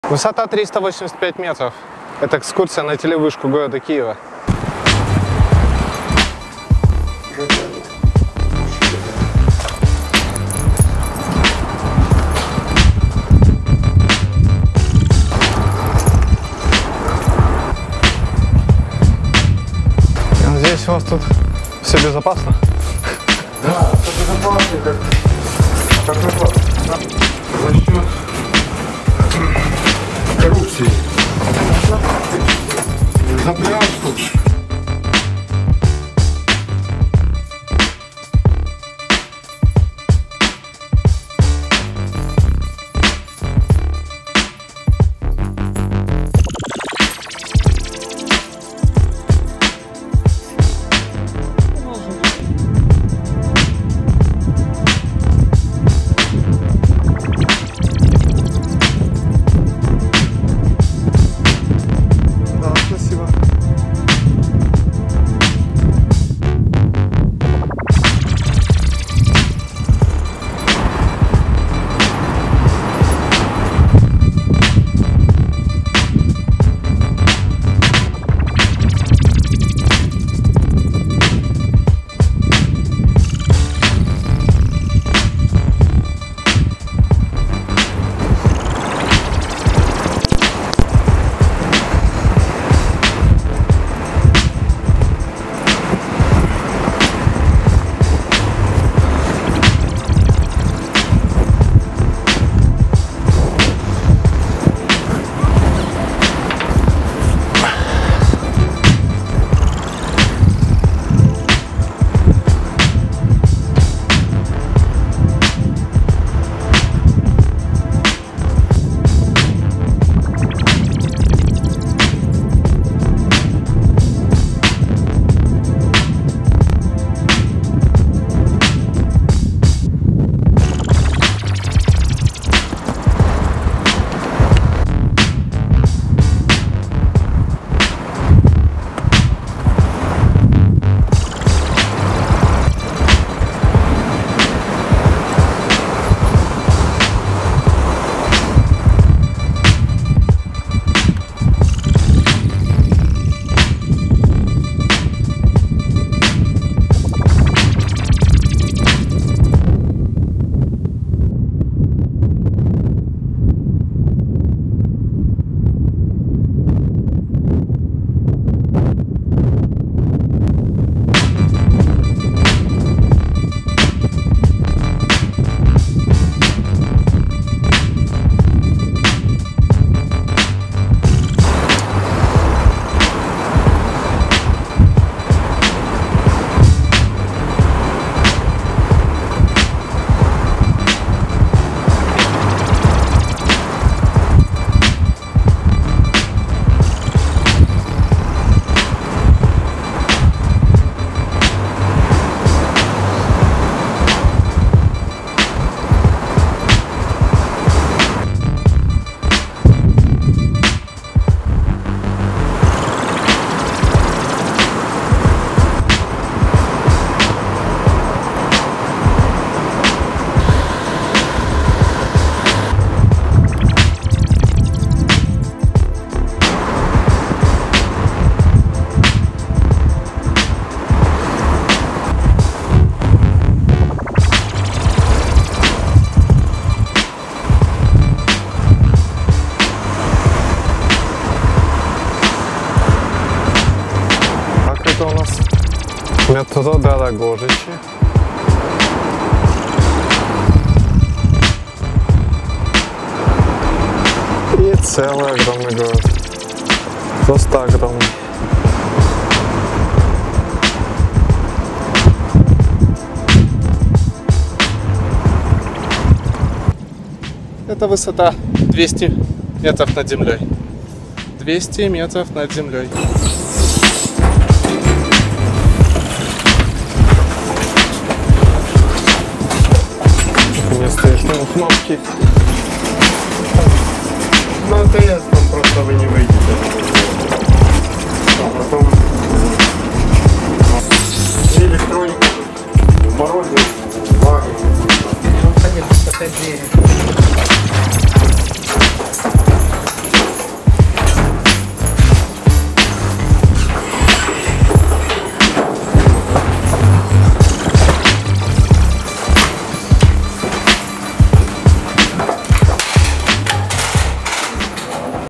Киевская телебашня. Телевышка
Краткая экскурсия на телебашню киева и обзор города с высоты 200м.